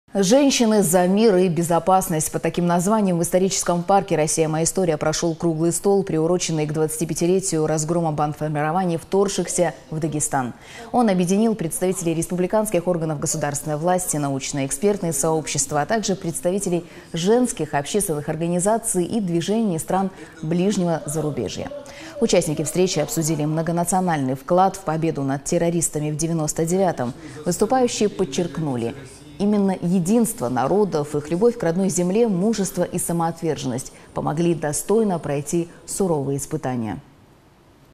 Круглый стол, приуроченный к 25-летию разгрома бандформирований, прошел в Махачкале
«Женщины за мир и безопасность». Под таким названием в историческом парке «Россия – моя история» прошел круглый стол, приуроченный к 25-летию разгрома бандформирований, вторгшихся в Дагестан. Он объединил представителей республиканских органов государственной власти, научно-экспертные сообщества, а также представителей женских общественных организаций и движений из стран ближнего зарубежья.